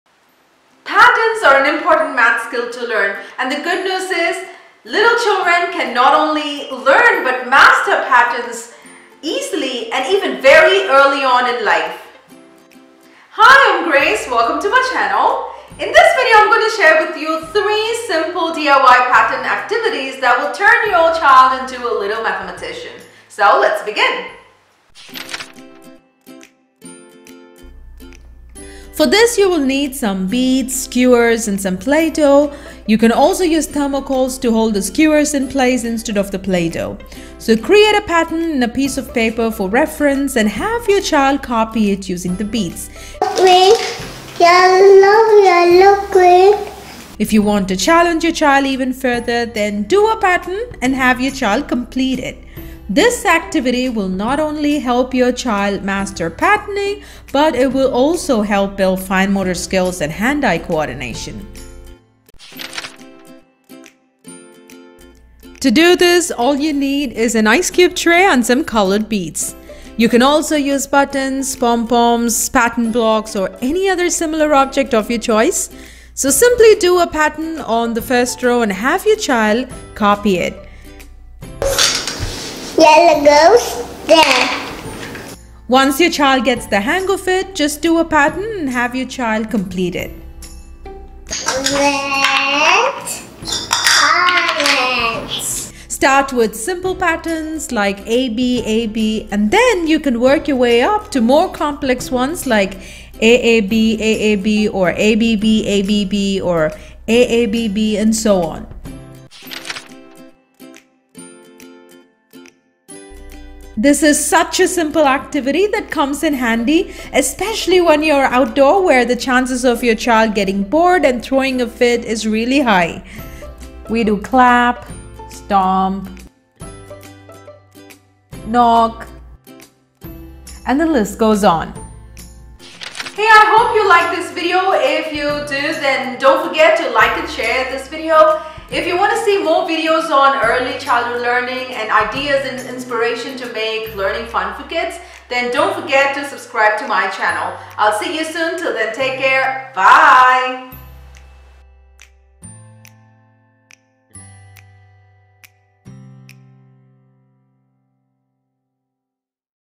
3 Pattern Activities That Will Make Your Child Smart!
Patterns are an important math skill to learn and the good news is children can not only learn but master patterns very early on in life.

In this video, I share 3 DIY pattern activities that our 2-year-old enjoys doing.

1. Pattern Threading
2. Pattern Copying
3. Pattern Imitation

Love,
Grace